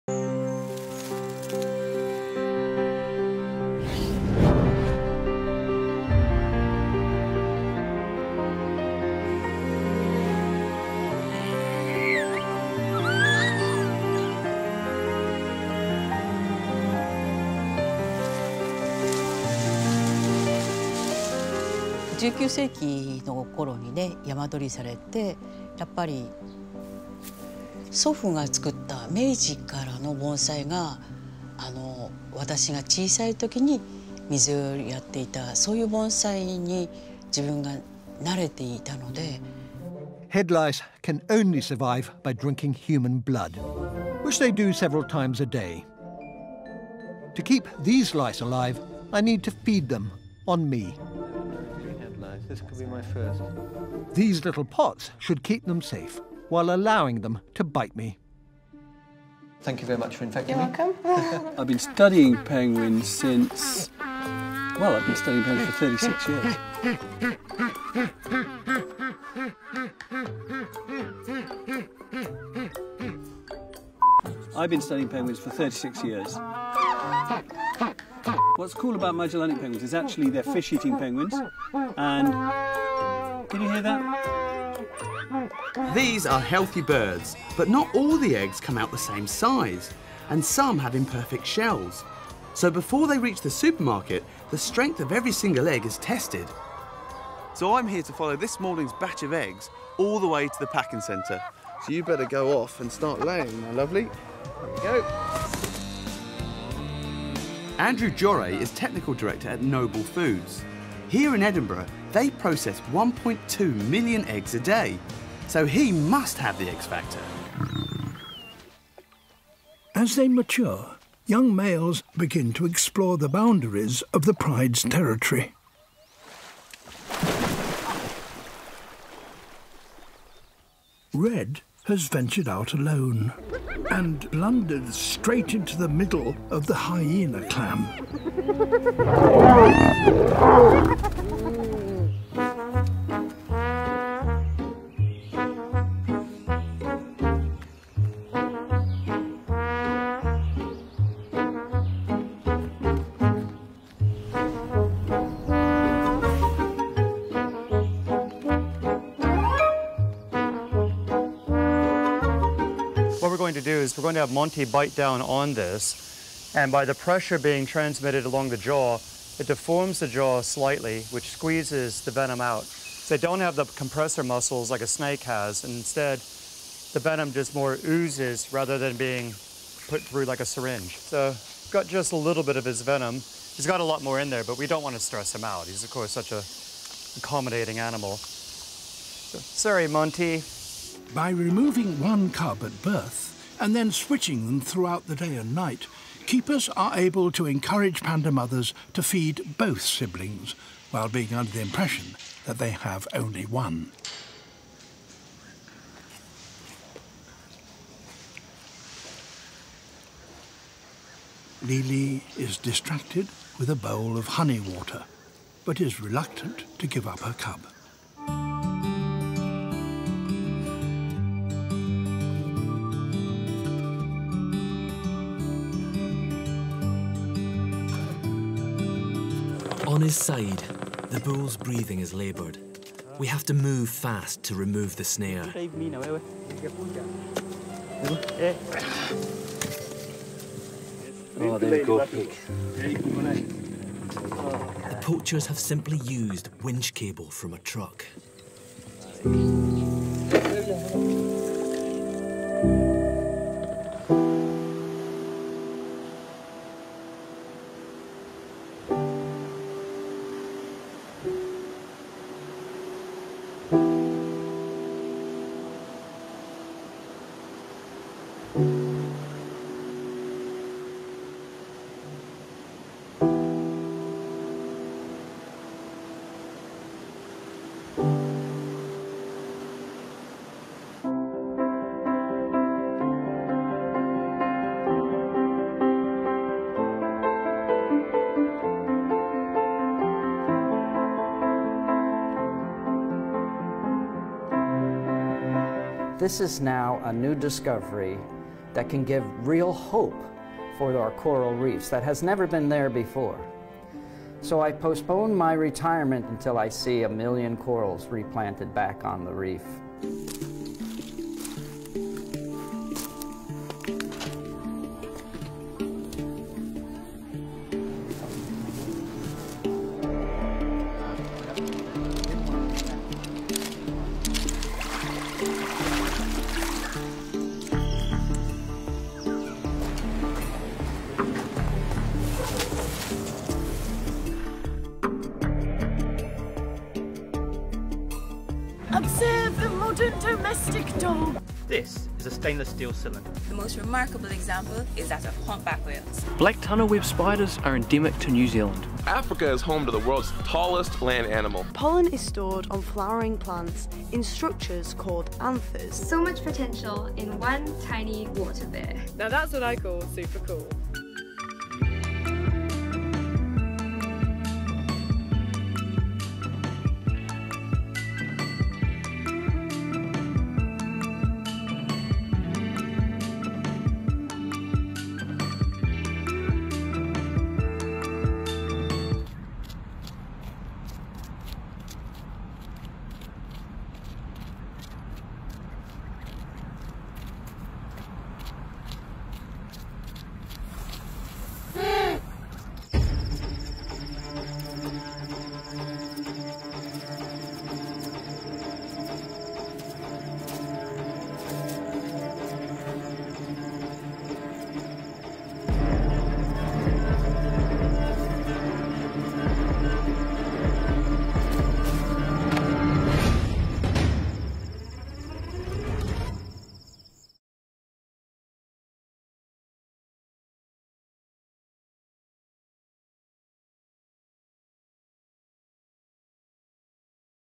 Best Moments of 2018 | BBC Earth
From the endless rituals of Bonsai making to the panda that didn't realise she had twins, passing through lions fighting hyenas and the cute pika, rediscover the best videos of 2018 from BBC Earth:
14. You were reminded to never work with animals
13. You were on the edge of your seat as a lion was surrounded by hyenas
7. You continued to be shocked by effects of plastic
6. You were inspired by the people fighting to save our planet
5. The world’s most trafficked animal broke you heart
4. Over 2000 of you applied to be our next digital presenter
3. You inspired us by sharing your world
2. You cried for baby penguins
1. Nearly 80m of you watched Africa’s smallest cat